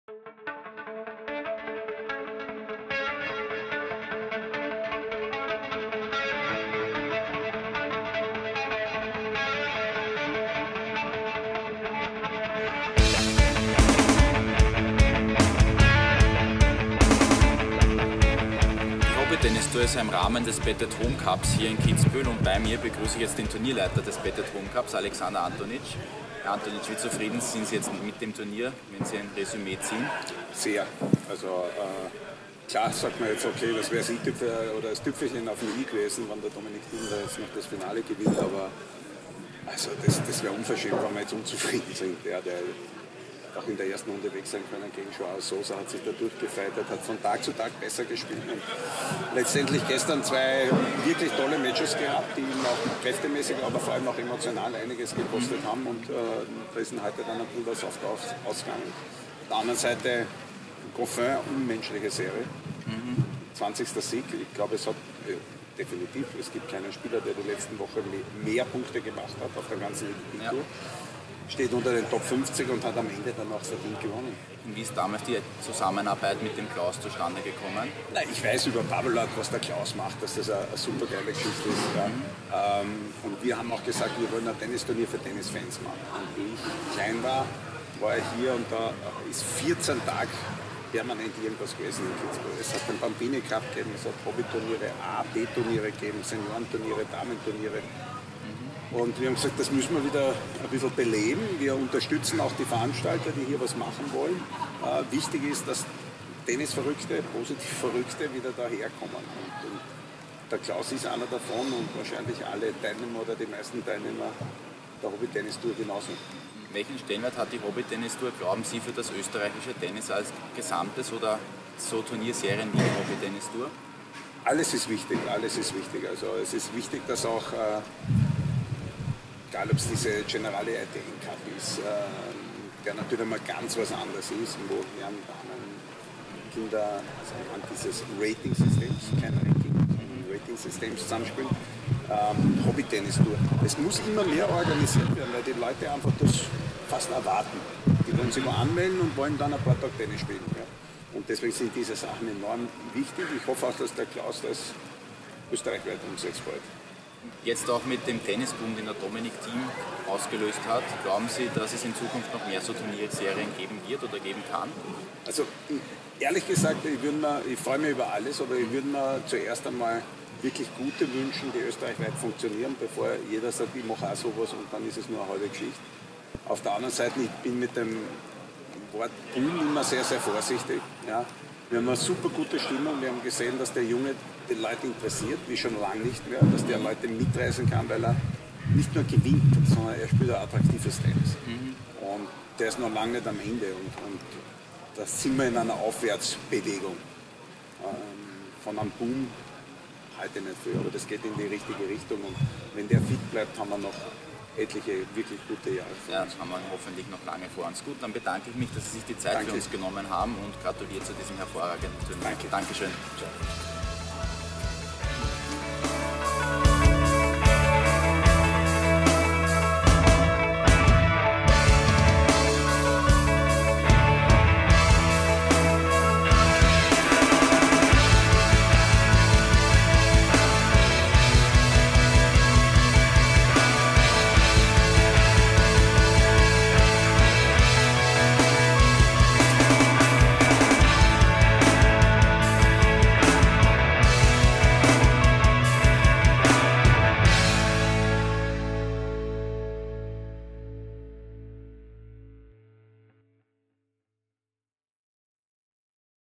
Kitzbühel Streifzüge 2014 - Alexander Antonitsch
Das Interview mit Alexander Antonitsch, dem Turnierdirektor des bet-at-home Cup Kitzbühel.

(apologize for the poor audio and the wind interferences)